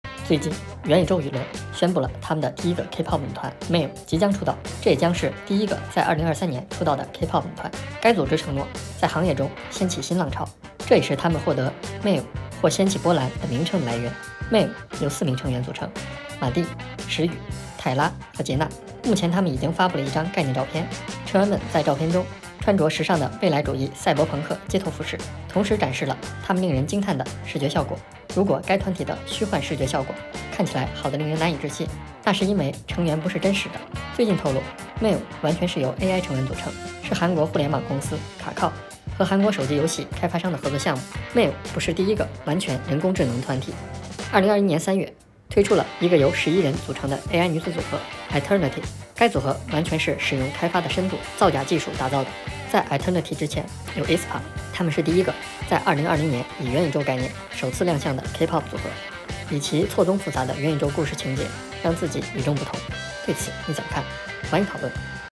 2023 年第一個 K-Pop 女團的概念引起了網友的不同反應
2023 年第一個 K-Pop 女團的概念引起了網友的不同反應#mave